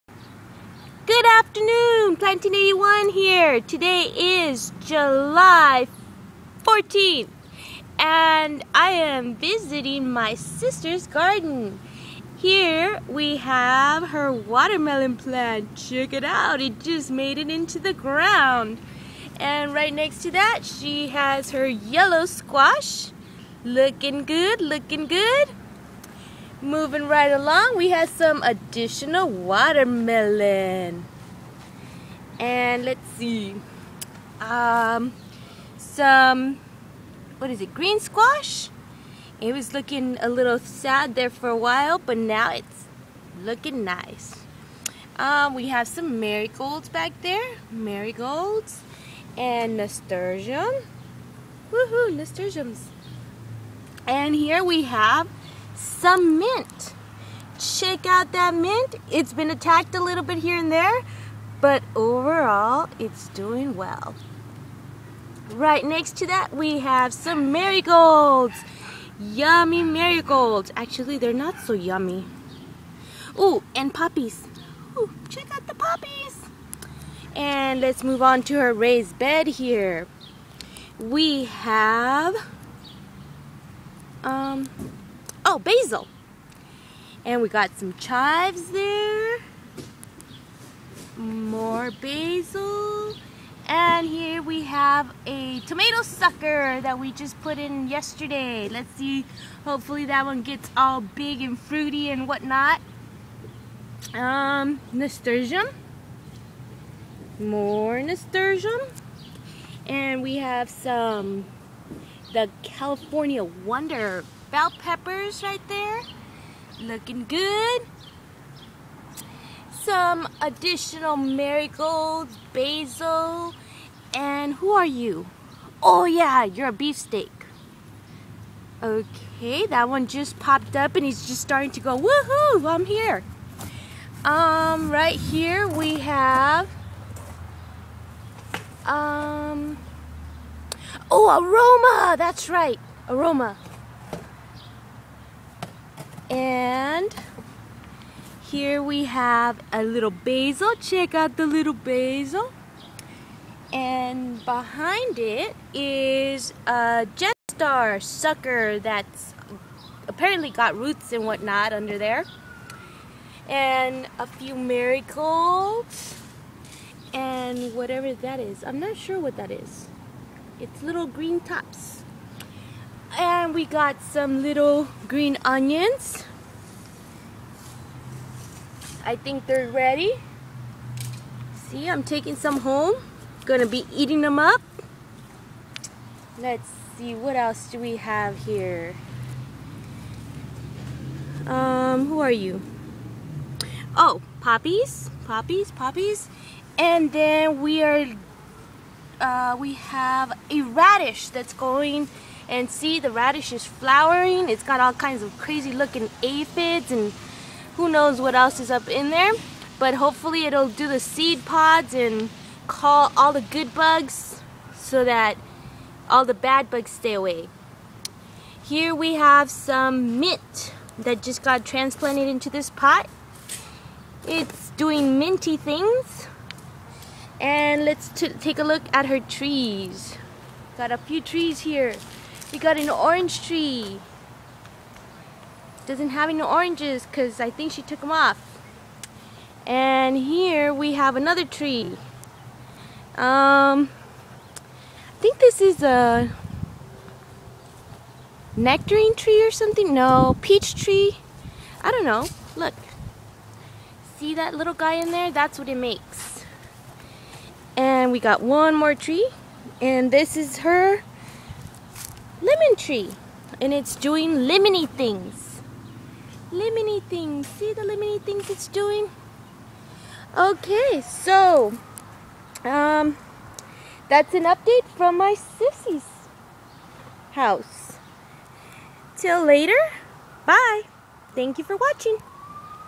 My sisters garden 07-14-11
Visiting my sister leads to an unexpected garden video.